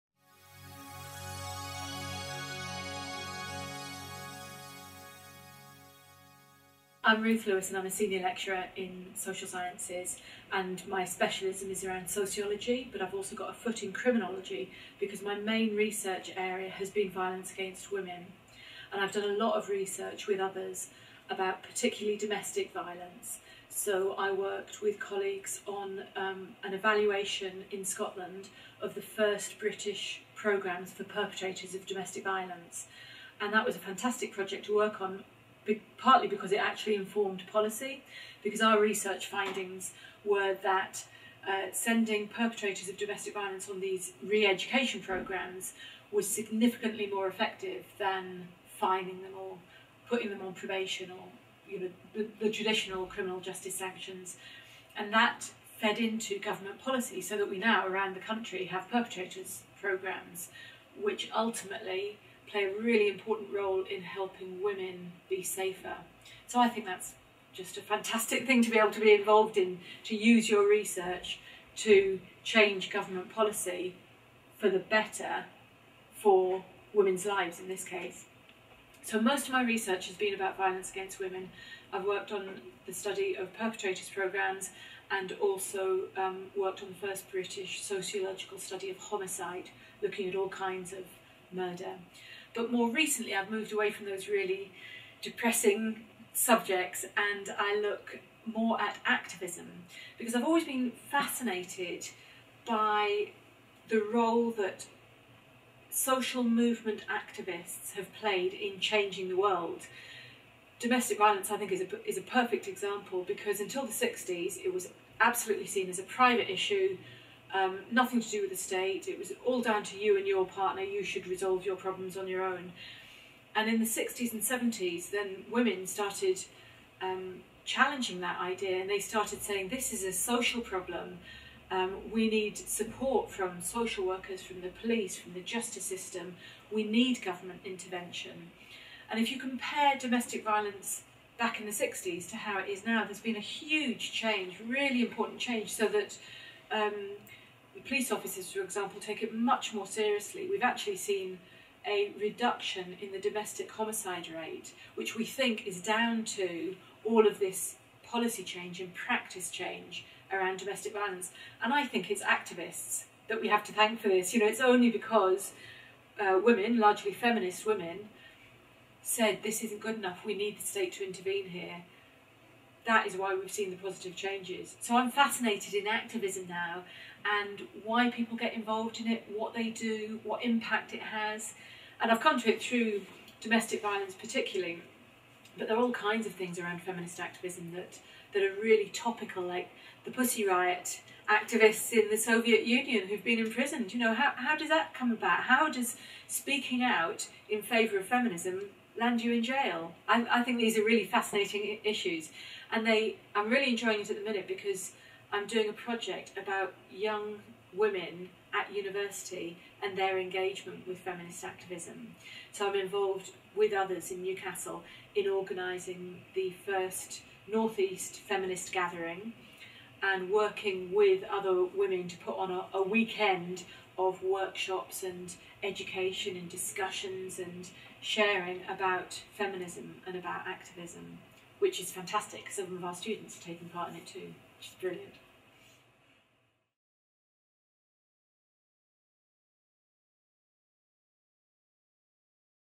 Researching Domestic Violence
In this video, Ruth Lewis (Northumbria University) talks about her research about domestic violence and feminist activism. Take a look at Northumbria University's Sociology webpages